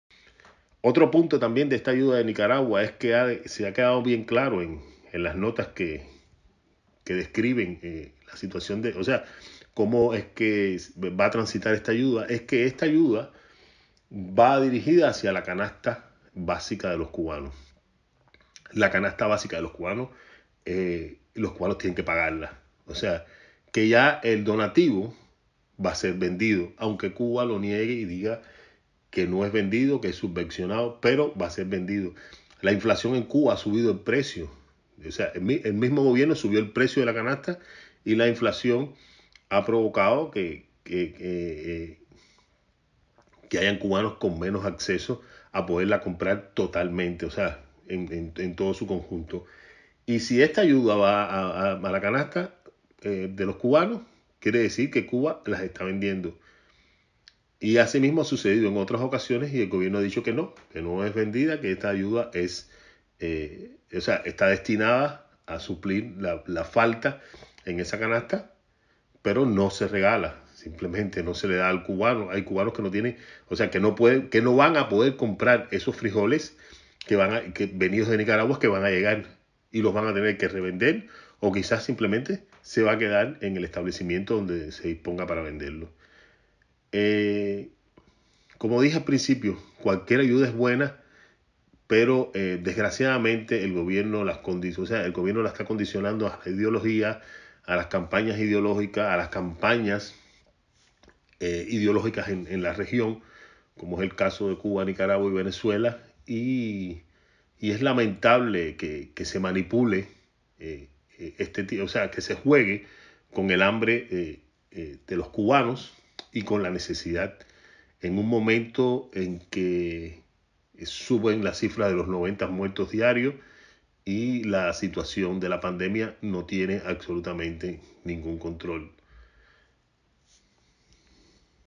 Cubanos pagarán "donación" de frijoles y aceite enviados de Nicaragua
Cubanos pagarán "donación" de frijoles y aceite enviados de Nicaragua, según el periodista  Augusto César Martin, de Cubanet.